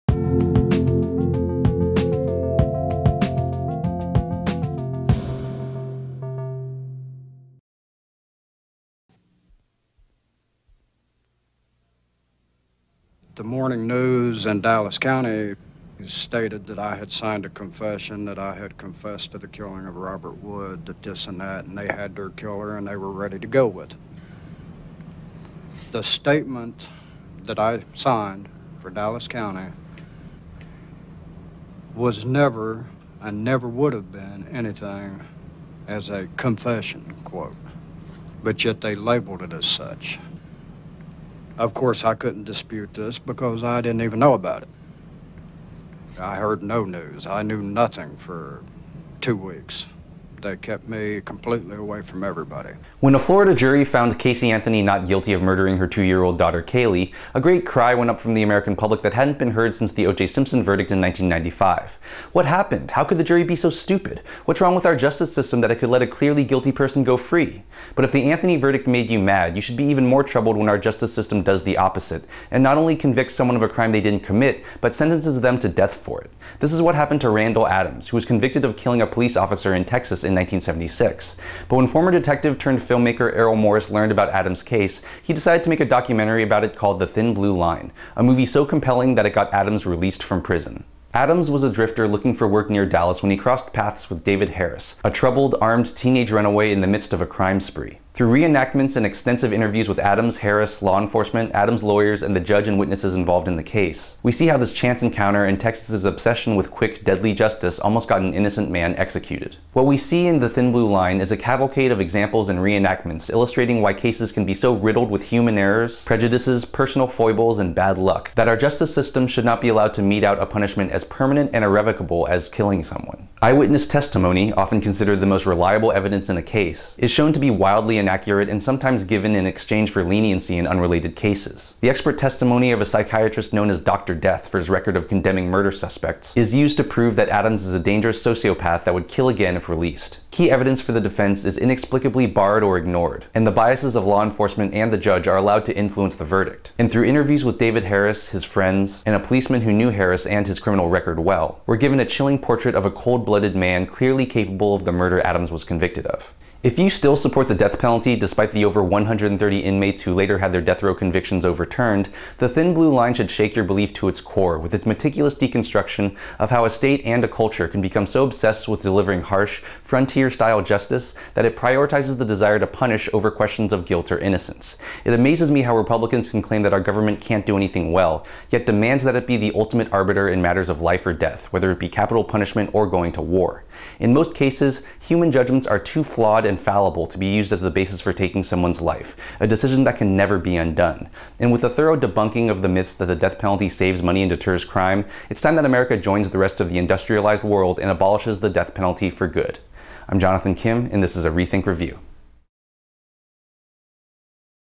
The Thin Blue Line - Movie Review by Jonathan Kim
Check out Jonathan Kim's review of Errol Morris' documentary THE THIN BLUE LINE, which is considered one of the best documentaries ever made. It tells the true story of Randall Adams, who was wrongly convicted of killing a police officer in Texas in 1976. THE THIN BLUE LINE did such a good job of discrediting the case against Adams that it led to Adams being released from prison.

Check out Jonathan Kim's new Netflix-based movie review and political show, ReThink Reviews on the TYT Network!

Errol Morris's unique documentary dramatically re-enacts the crime scene and investigation of a police officer's murder in Dallas, Texas. Briefly, a drifter (Randall Adams) ran out of gas and was picked up by a 16-year-old runaway (David Harris). Later that night, they drank some beer, smoked some marijuana, and went to the movies. Then, their stories diverged. Adams claimed that he left for his motel, where he was staying with his brother, and went to sleep. Harris, however, said that they were stopped by police late that night, and Adams suddenly shot the officer approaching their car. The film shows the audience the evidence gathered by the police, who were under extreme pressure to clear the case. It strongly makes a point that the circumstantial evidence was very flimsy. In fact, it becomes apparent that Harris was a much more likely suspect and was in the middle of a crime spree...